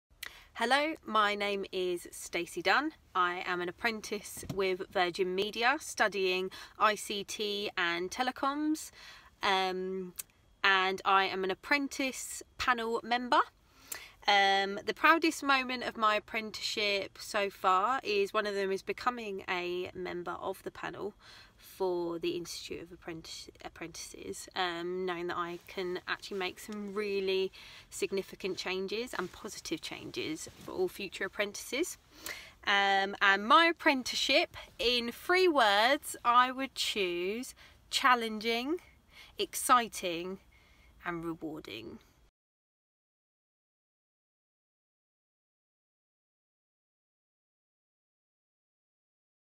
Stacey Dunne- Virgin Media
Our apprentice panel tells us about their proudest moment being an apprenticeship and three words to describe their apprenticeship.

You can find more information about the apprentice panel on the Institute’s website.